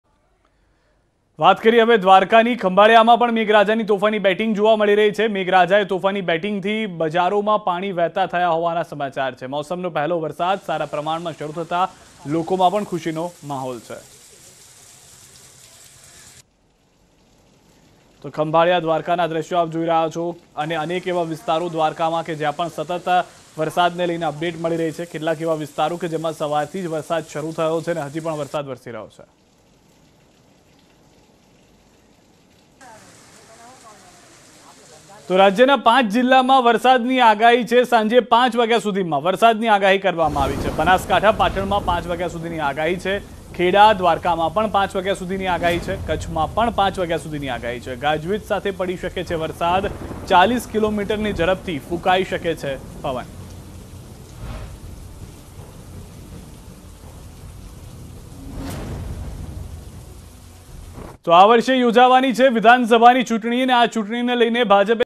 Dwarka Weather News : Dwarka માં જોવા મળી મેઘરાજાની મેઘસવારી | Monsoon 2022 | News18 Gujarati
Dwarka માં જોવા મળી મેઘરાજાની મેઘસવારી. Khambhalia માં રસ્તા પર વહેતા થયા પાણી. વરસાદથી ખેડૂતોમાં ખુશીનો માહોલ.